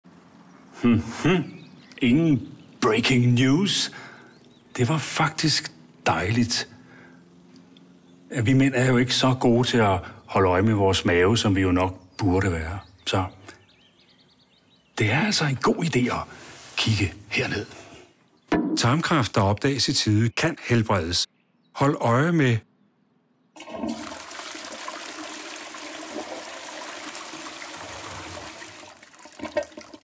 Per Christiansen - hold øje mand!
Tarmkræft der opdages i tide kan helbredes, så husk at holde øje.